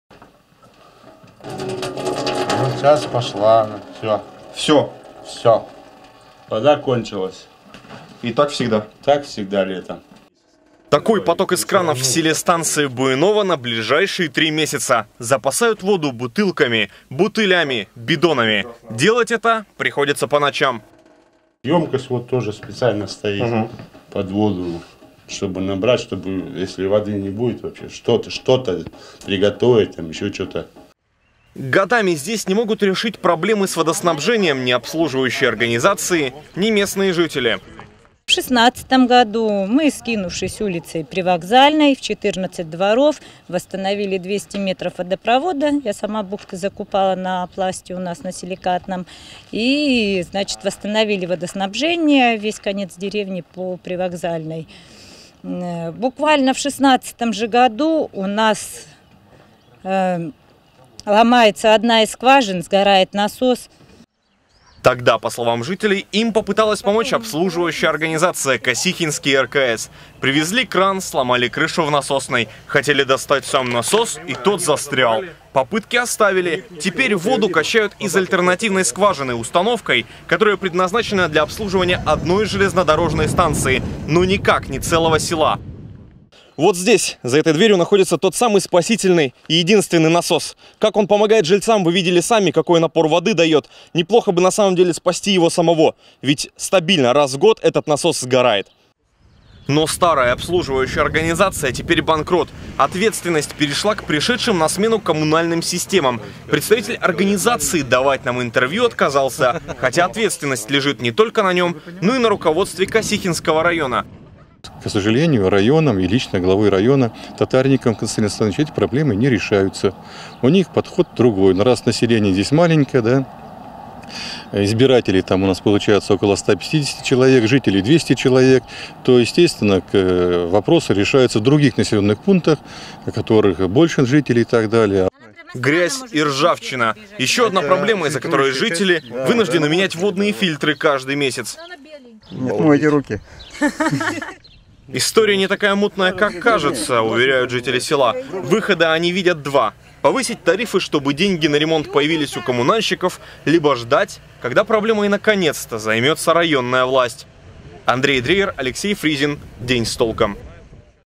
Жители станции Баюново ждут воду в кранах уже несколько лет
В одном из сел Косихинского района годами не могут наладить водоснабжение. У жителей села Станция Баюново воды в кранах практически не бывает, а та, что иногда появляется, непригодна для питья. Предыдущая обслуживающая организация «Косихинский РКС» попыталась поменять насос в водоносной скважине, но только сломала действующий. А теперь решать эту проблему предстоит новой компании.